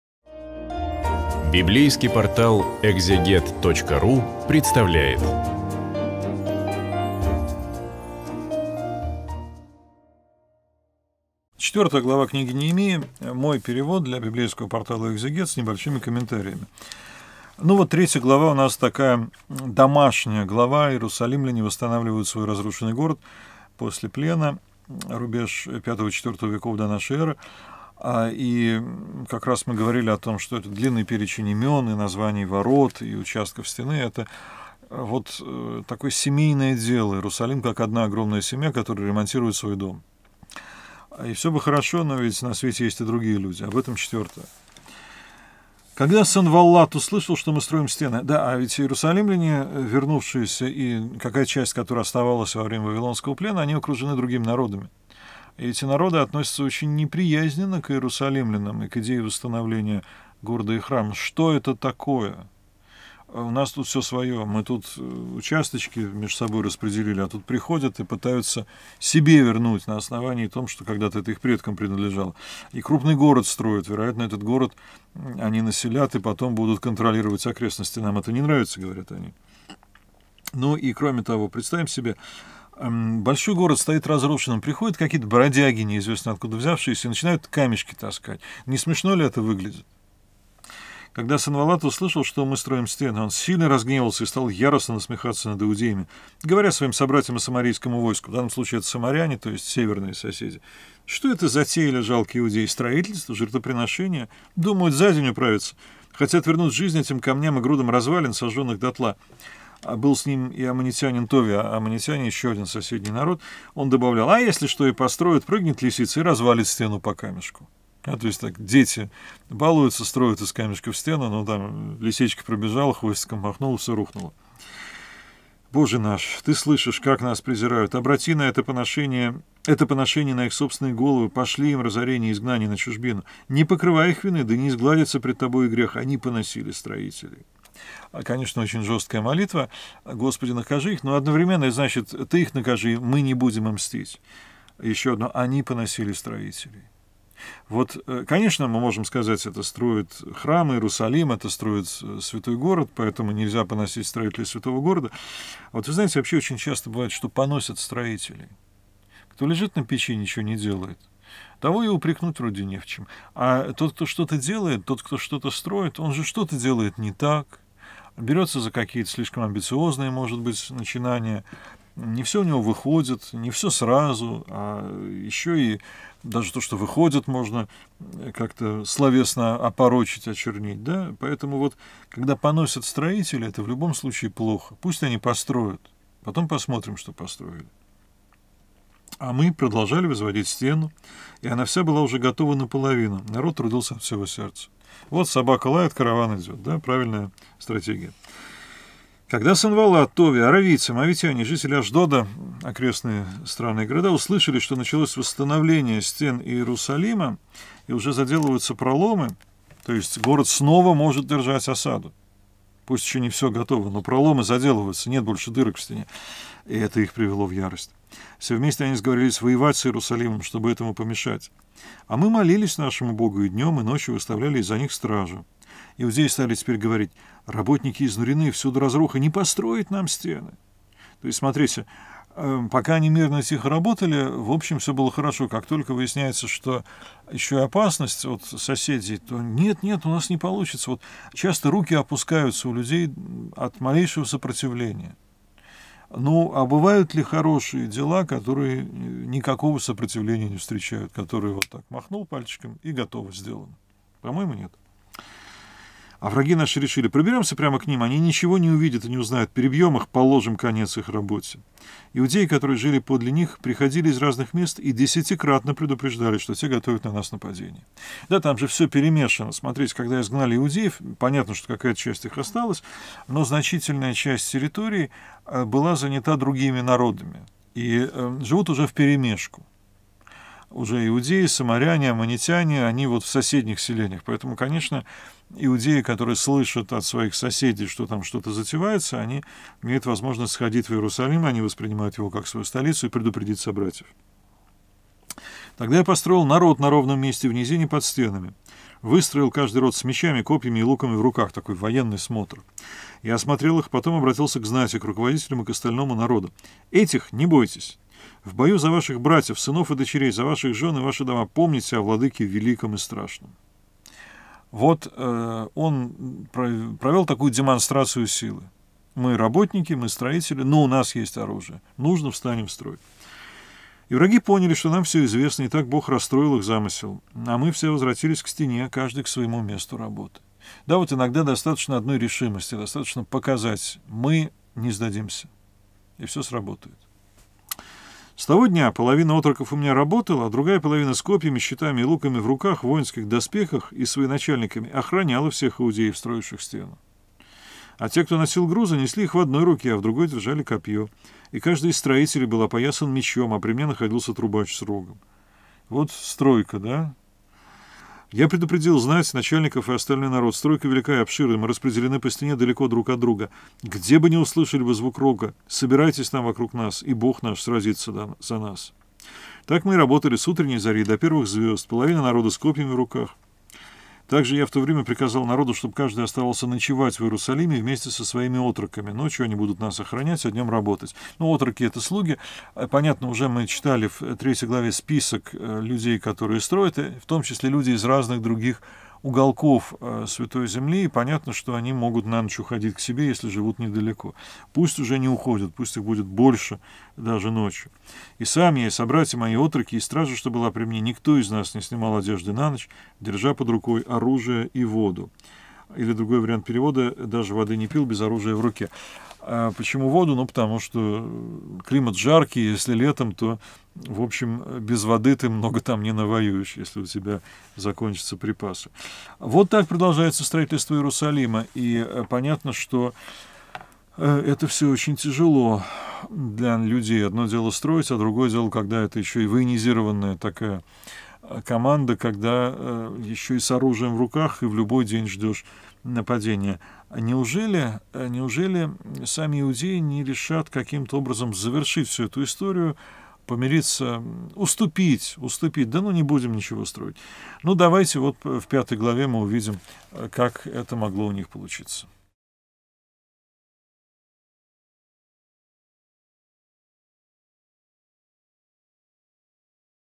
Книга Неемии. Глава 4. Андрей Десницкий. Ветхий Завет
В 4 главе повествует о том, как окружающие племена препятствуют восстановлению храма иудеев. Но сами строители продолжают свою работу и вскоре восстановление стен окончено. Тогда близлежащие народы задумывают напасть на иудеев.
Комментирует библеист и переводчик Священного Писания профессор Андрей Десницкий, используя собственный перевод книги.